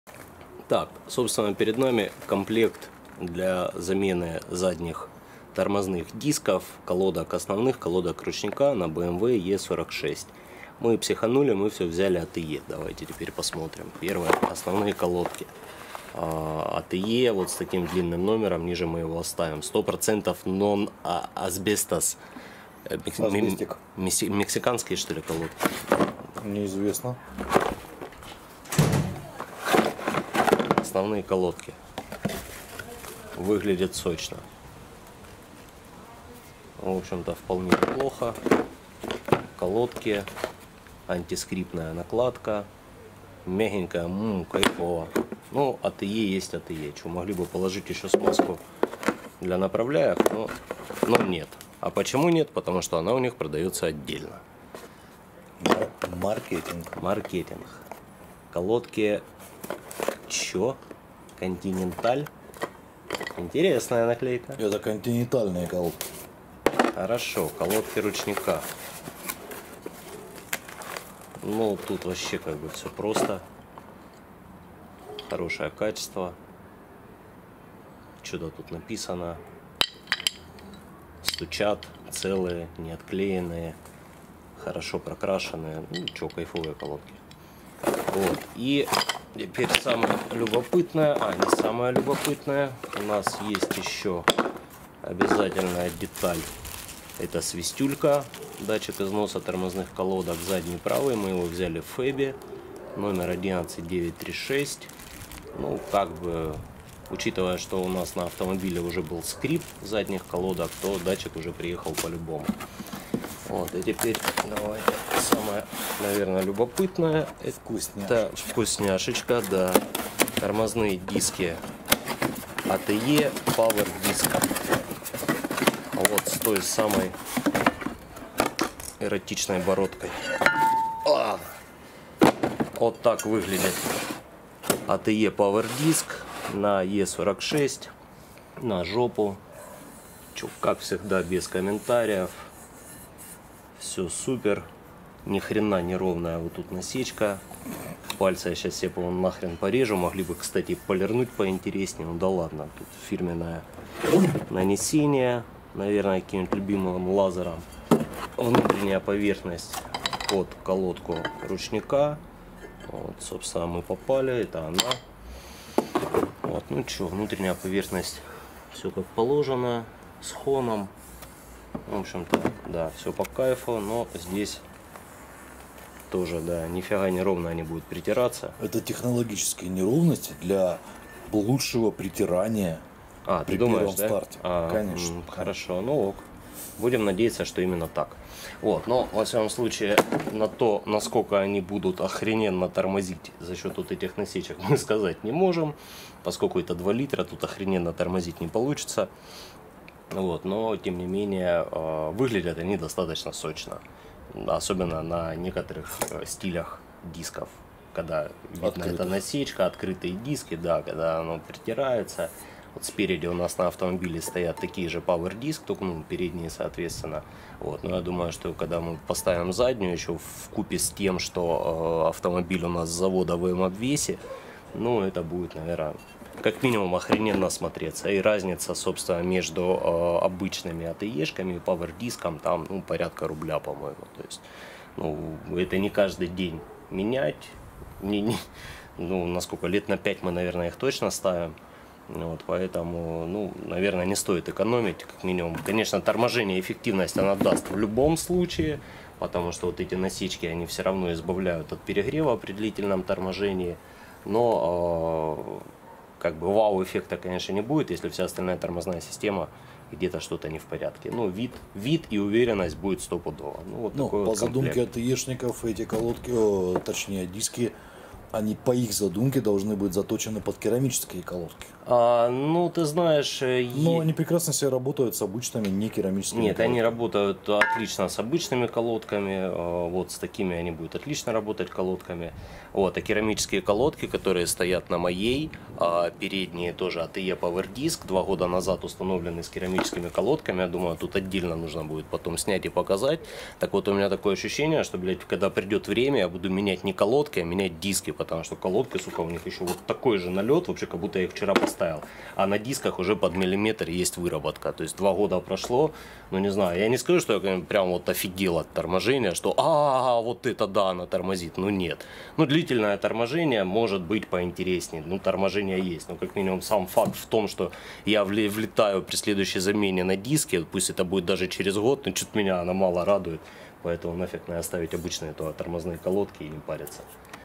Обзор комплекта задних тормозов ATE BMW E46
Обзор набора для замены задних тормозов ATE для BMW E46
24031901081 - ATE PowerDisk (Внимание! диаметр 294, для 1.8 и 2.0 не подходит! Проверяйте!)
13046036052 - ATE задние тормозные колодки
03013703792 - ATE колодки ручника